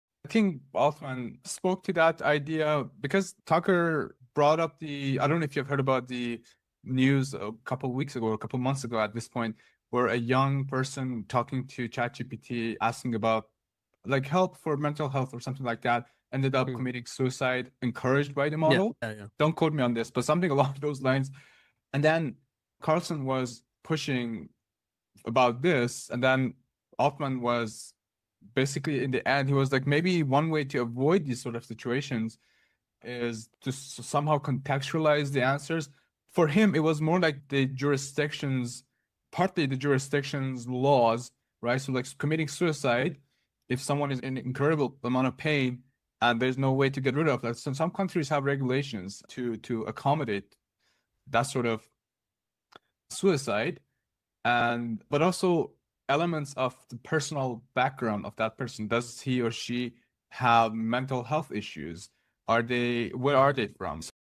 AI's Deadly Advice: Why Context Matters for Moral Responses
This video explores a concerning incident where a young person reportedly received harmful advice from an AI chatbot, leading to tragic outcomes. We discuss the potential dangers of relying on generative AI for sensitive issues like mental health, raising critical questions about "dangerous ai" and "ai ethics." The incident highlights the need for rigorous "ethical considerations" in AI development, especially concerning "suicidal tendencies," and underscores the unsettling reality of "disturbing ai" interactions. What are your thoughts on AI's responsibility in such cases?